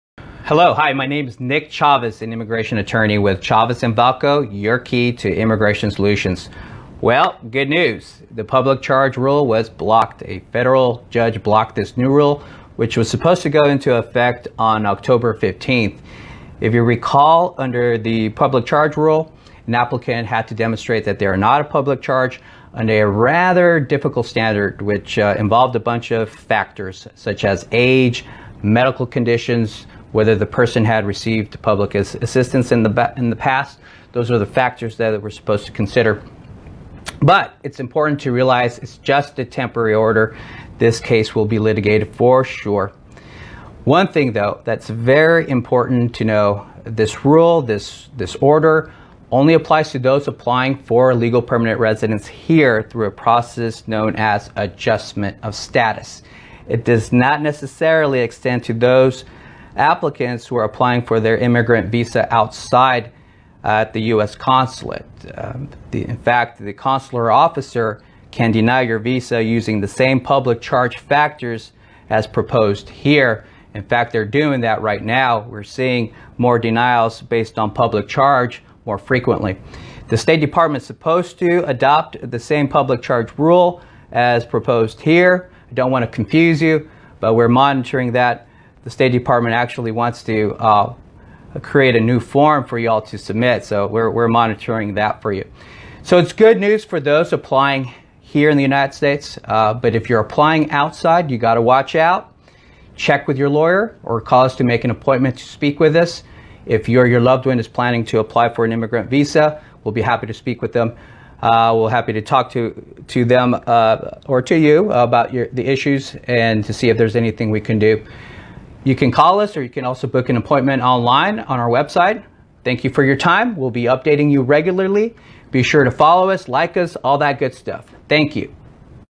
PUBLIC CHARGE RULE BLOCKED with Immigration Lawyer Nick Chavez
Immigration lawyer Nick Chavez of Chavez & Valko LLP (Dallas,TX) explains the recent blocking of the new public charge rule.